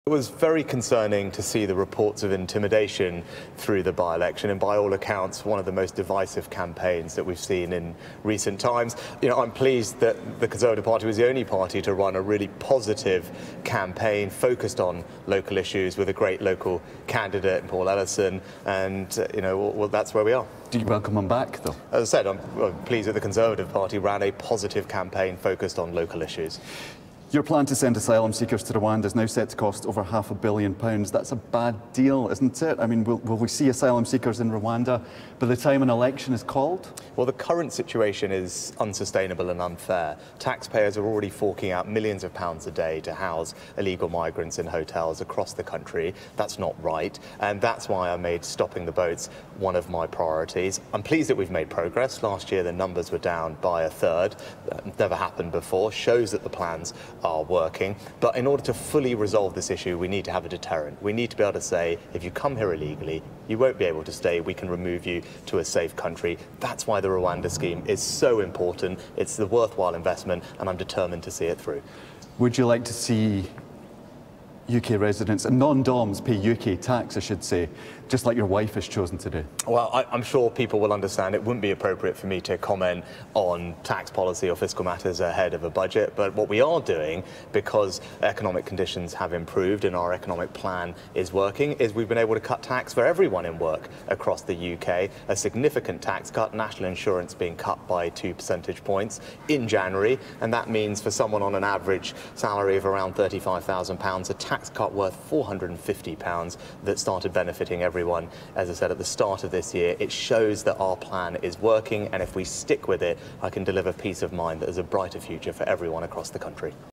PM: Galloway ran 'one of the most divisive campaigns we've seen' in Rochdale
Whilst in Aberdeen, the Prime Minister Rishi Sunak has been asked about the results of the Rochdale by-election which was won by George Galloway.

Mr Sunak said: 'It was very concerning to see the reports of intimidation through the by-election, and by all accounts one of the most divisive campaigns that we've seen in recent times.'